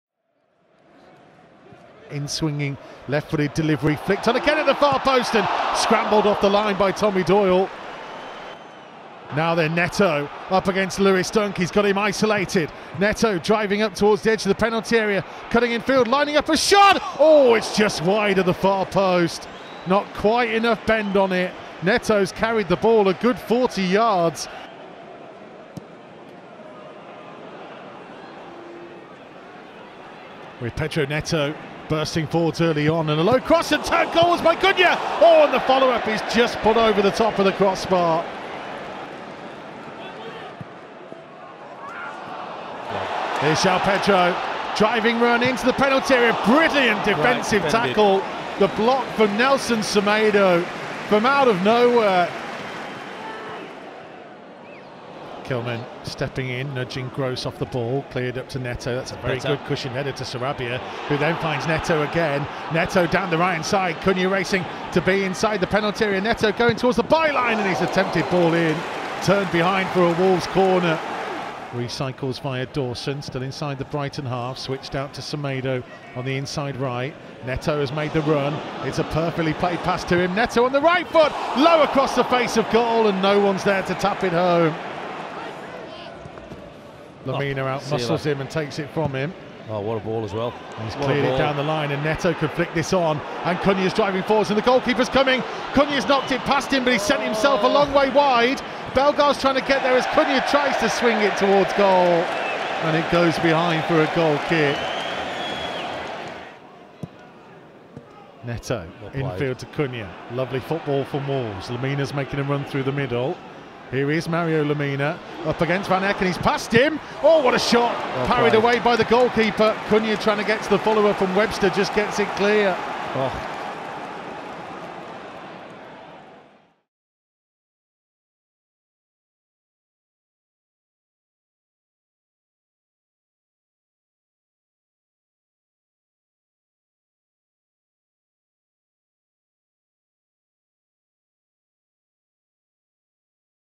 Stalemate with the Seagulls | Brighton 0-0 Wolves | Highlights
A hard-fought point on the south coast for Gary O'Neil's side as it ends goalless with Brighton. An even game with the home side seeing a lot of the ball, but Wolves were dangerous on the counter, with Pedro Neto, Matheus Cunha and Mario Lemina all threatening the Brighton goal.